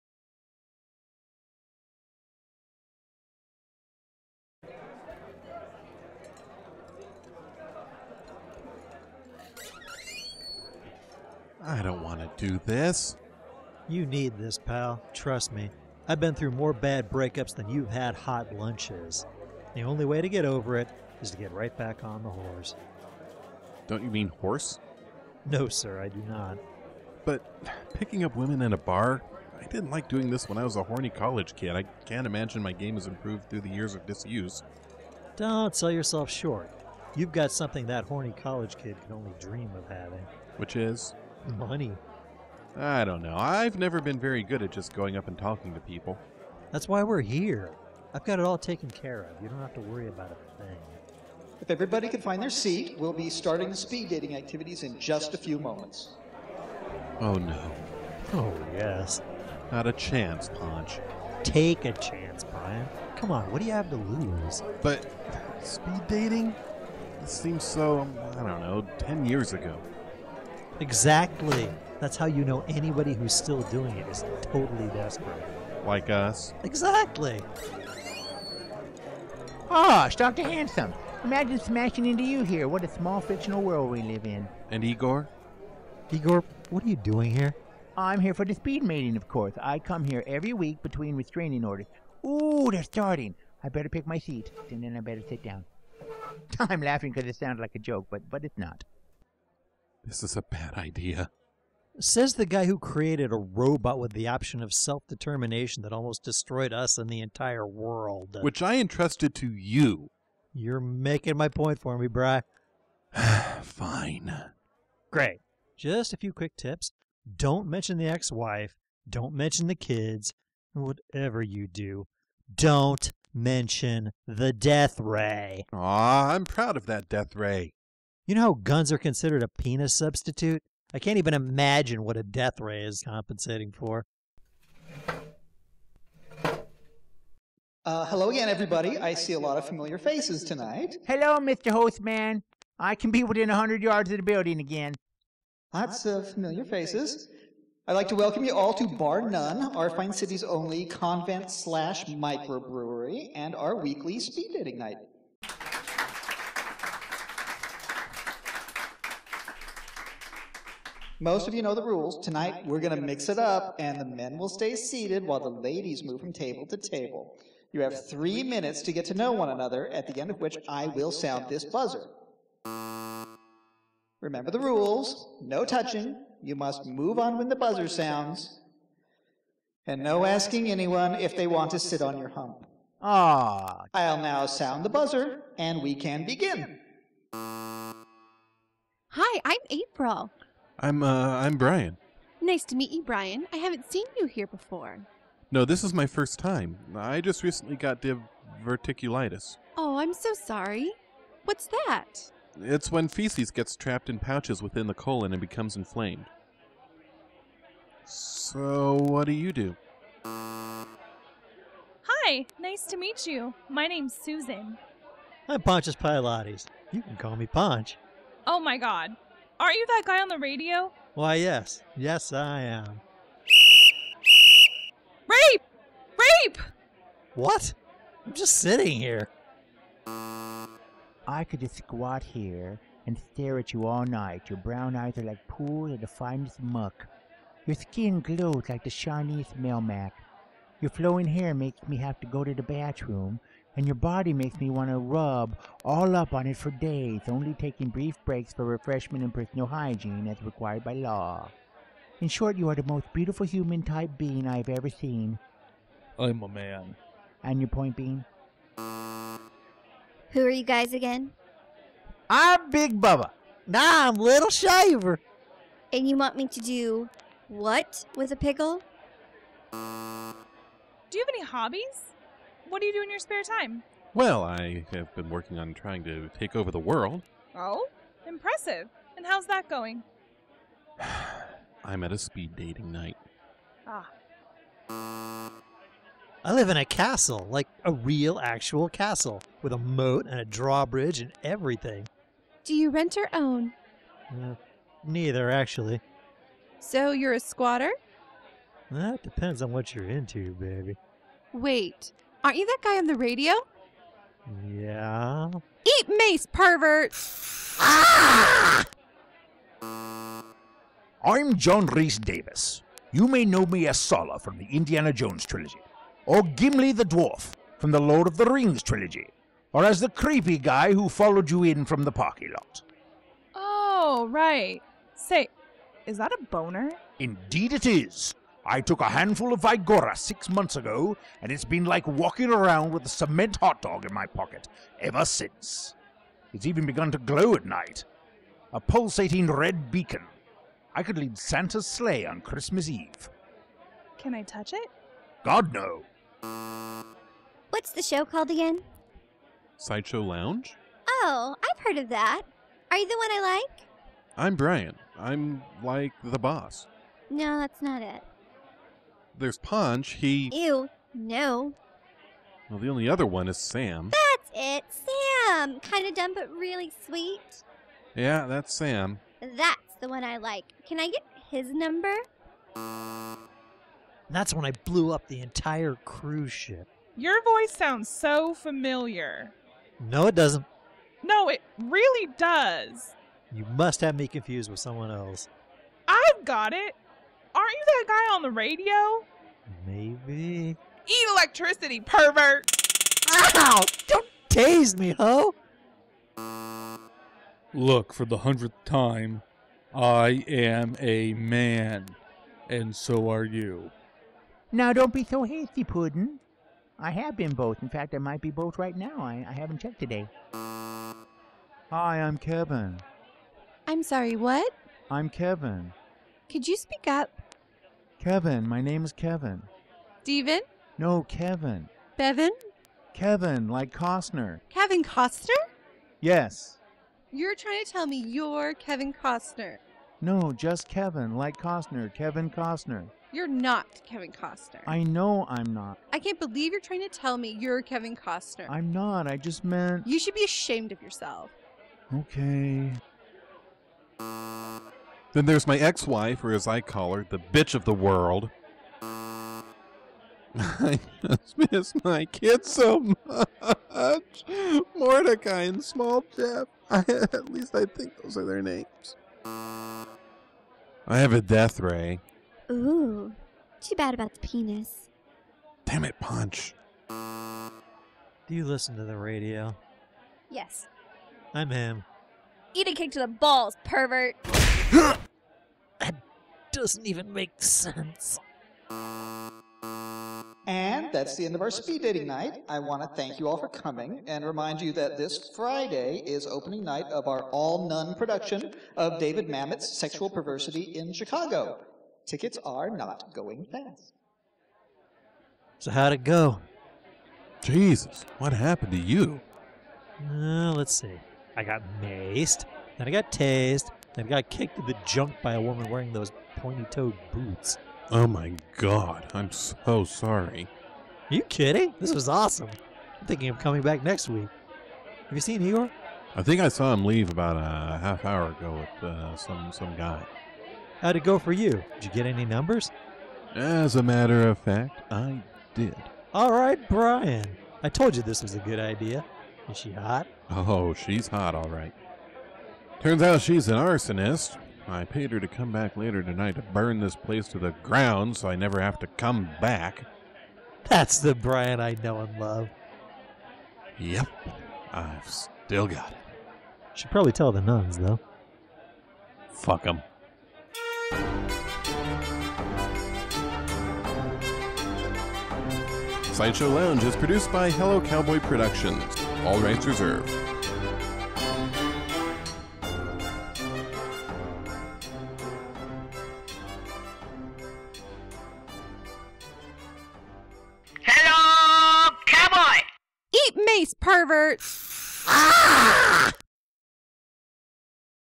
SideShow LOUNGE - Episode 044 "Speed Dating Night In The Garden Of Good And Evil"
IN THIS EPISODE: It's time to get back out there for Brian.